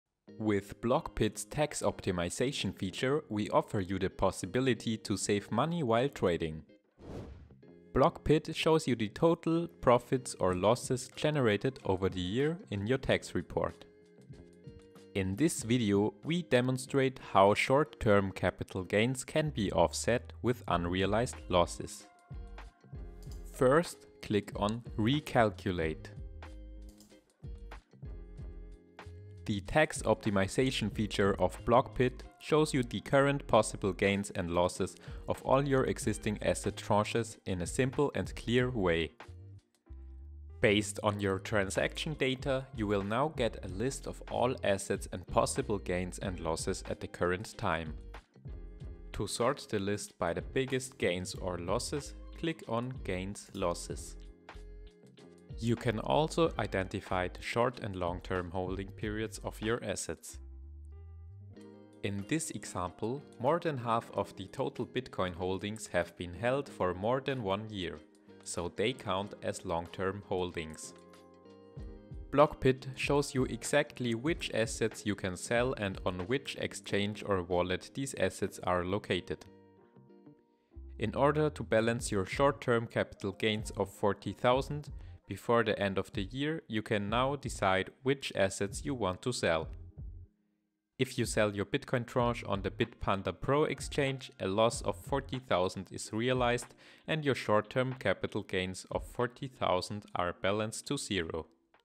Crypto Tax Optimization - Blockpit Tutorial 2023
The Tax Optimization Feature allows you to identify current gains and losses of all your existing asset tranches and their holding period.

Depending on your tax country, it is possible to realize gains as well as losses in order to optimize your tax burden within the respective year (offsetting losses against gains).

In the US, for example, a sale of a tranche that has been held for more than 365 days falls under long-term capital gains and is taxed differently than short-term capital gains.

Our Tax Optimization Feature provides the information you need to optimize your taxes!